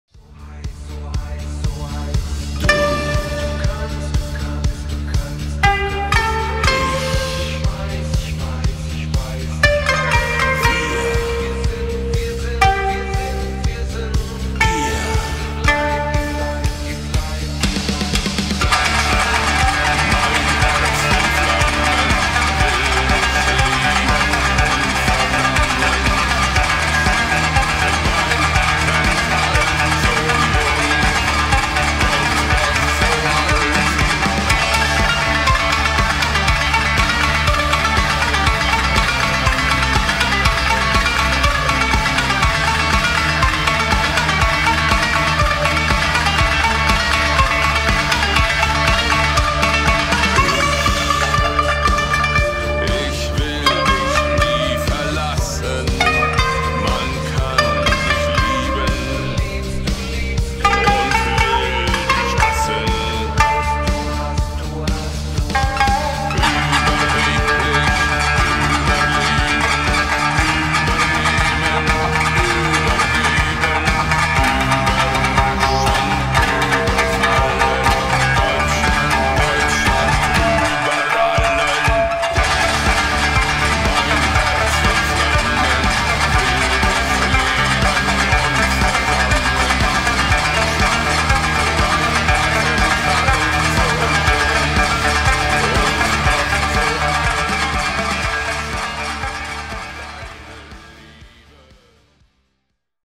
Short Rammstein cover on Shamisen
What can I say? When I heard that phrase, I could only think of shamisen twanging away in it too!